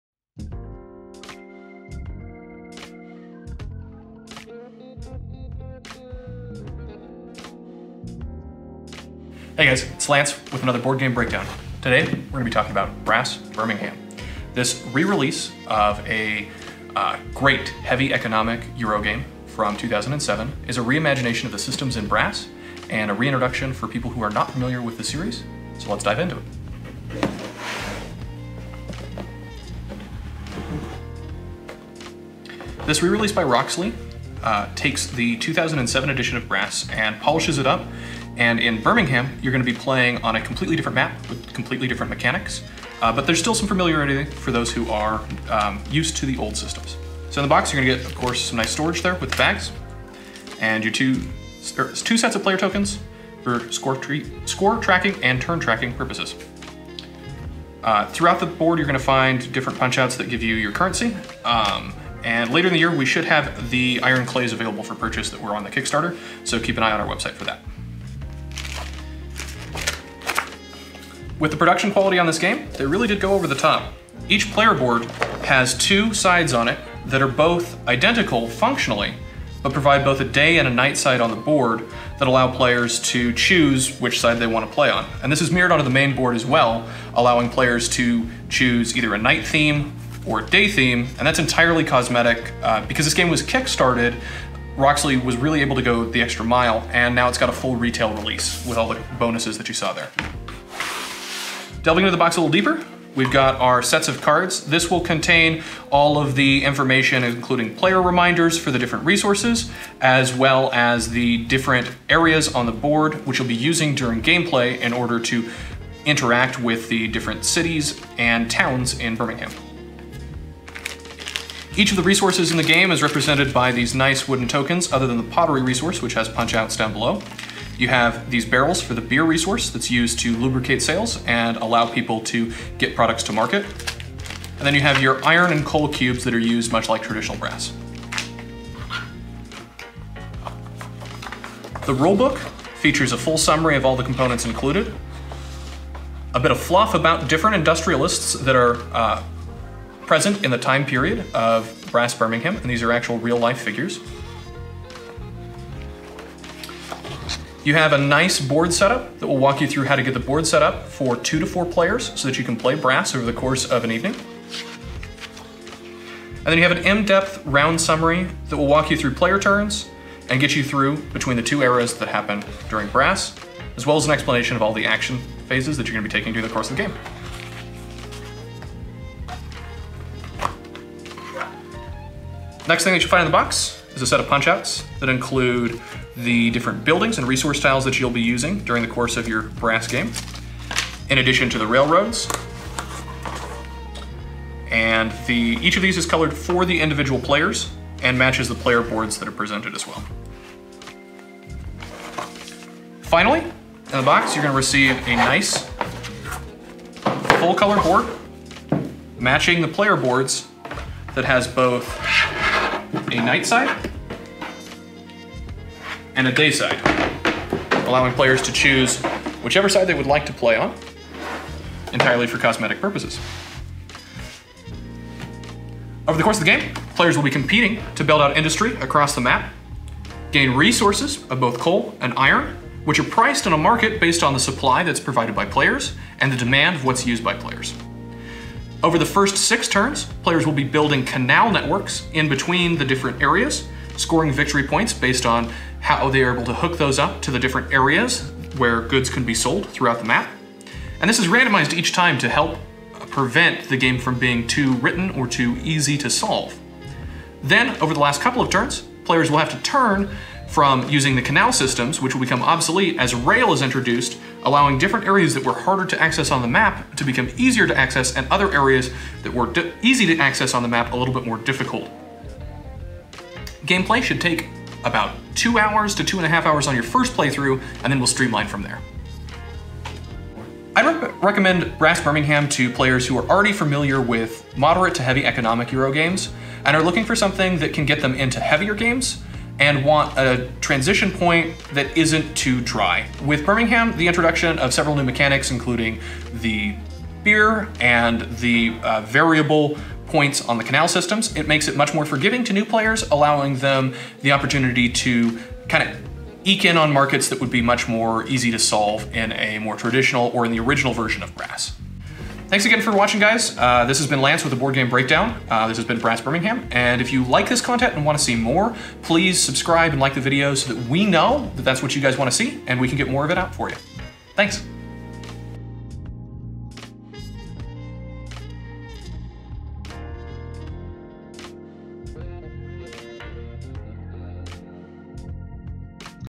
Brass: Birmingham Boardgame Breakdown with Lance from ABU Games
You know who is excited about Brass: Birmingham? US! We're so stoked to have this in stock! Follow along with our main man Lance as he walks us through what we can expect to find inside this awesome box. What do you think? Are you as pumped for this game as we are?

From the Publisher:

"Brass: Birmingham is an economic strategy game sequel to Martin Wallace' 2007 masterpiece, Brass. Birmingham tells the story of competing entrepreneurs in Birmingham during the industrial revolution, between the years of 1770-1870.

As in its predecessor, you must develop, build, and establish your industries and network, in an effort to exploit low or high market demands.

Each round, players take turns according to the turn order track, receiving two actions to perform any of the following actions (found in the original game):

1) Build - Pay required resources and place an industry tile.
2) Network - Add a rail / canal link, expanding your network.
3) Develop - Increase the VP value of an industry.
4) Sell - Sell your cotton, manufactured goods and pottery.
5) Loan - Take a £30 loan and reduce your income.

Brass: Birmingham also features a new sixth action:

6) Scout - Discard three cards and take a wild location and wild industry card. (This action replaces Double Action Build in original Brass.)

The game is played over two halves: the canal era (years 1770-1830) and the rail era (years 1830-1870). To win the game, score the most VPs. VPs are counted at the end of each half for the canals, rails and established (flipped) industry tiles.

Birmingham features dynamic scoring canals/rails. Instead of each flipped industry tile giving a static 1 VP to all connected canals and rails, many industries give 0 or even 2 VPs. This provides players with the opportunity to score much higher value canals in the first era, and creates interesting strategy with industry placement.

Iron, coal, and cotton are three industries which appear in both the original Brass as well as in Brass: Birmingham.

New "Sell" system

Brewing has become a fundamental part of the culture in Birmingham. You must now sell your product through traders located around the edges of the board. Each of these traders is looking for a specific type of good each game. To sell cotton, pottery, or manufactured goods to these traders, you must also "grease the wheels of industry" by consuming beer. For example, a level 1 cotton mill requires one beer to flip. As an incentive to sell early, the first player to sell to a trader receives free beer.

Birmingham features three all-new industry types:

Brewery - Produces precious beer barrels required to sell goods.

Manufactured goods - Function like cotton, but features eight levels. Each level of manufactured goods provides unique rewards, rather than just escalating in VPs, making it a more versatile (yet potentially more difficult) path vs cotton.

Pottery - These behemoths of Birmingham offer huge VPs, but at a huge cost and need to plan.

Increased Coal and Iron Market size - The price of coal and iron can now go up to £8 per cube, and it's not uncommon.

Brass: Birmingham is a sequel to Brass. It offers a very different story arc and experience from its predecessor."